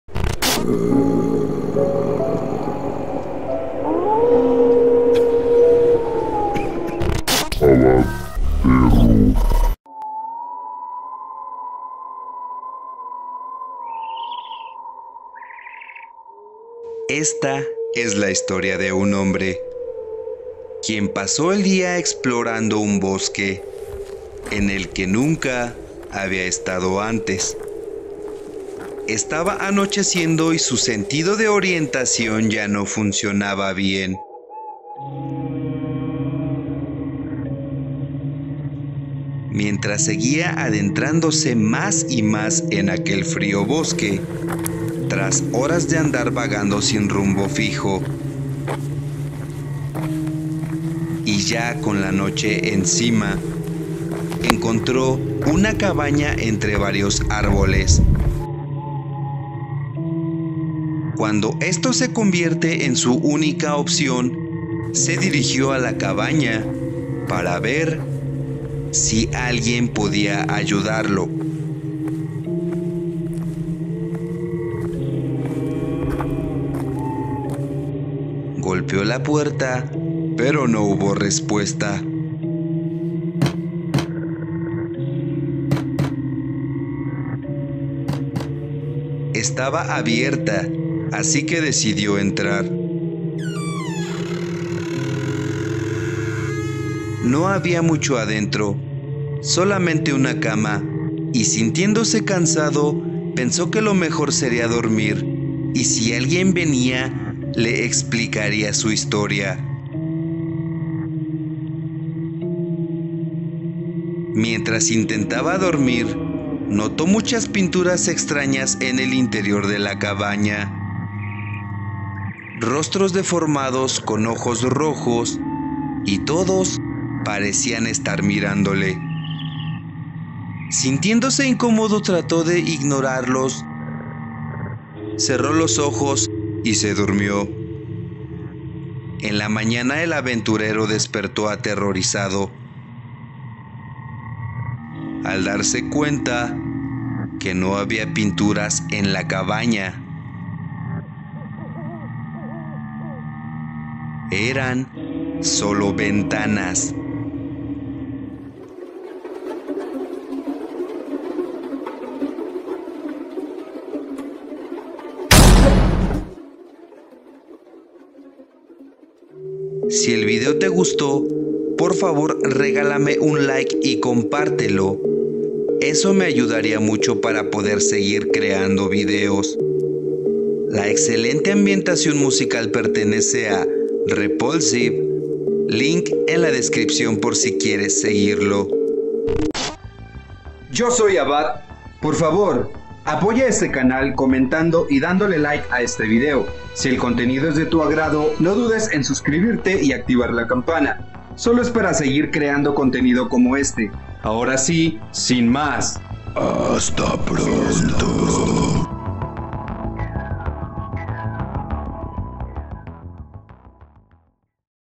ATERRADORAS Pinturas en La Cabaña del Bosque | ABBATH TERROR
ABBATH TERROR:
Las ATERRADORAS Pinturas de la Cabaña en medio del Bosque (Historia corta de Terror)